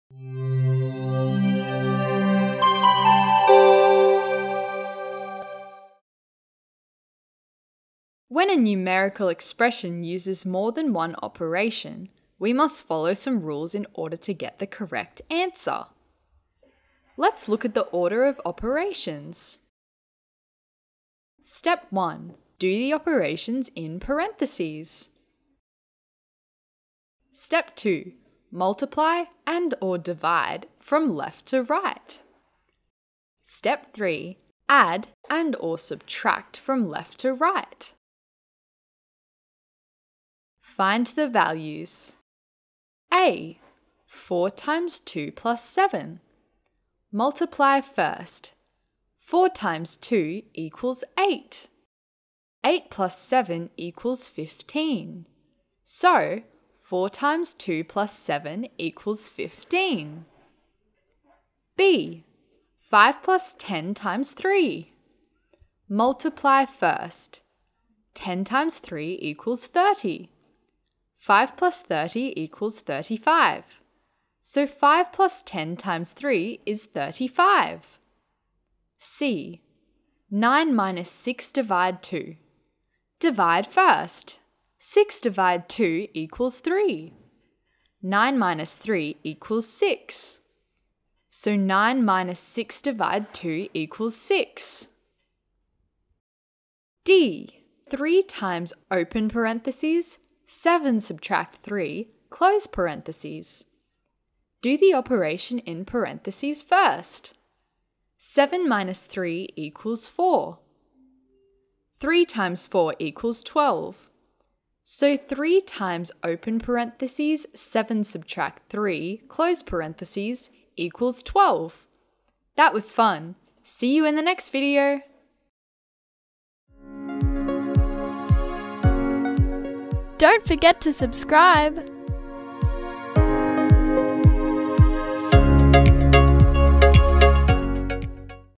Order of Operations
Matholia educational maths video on the order of operations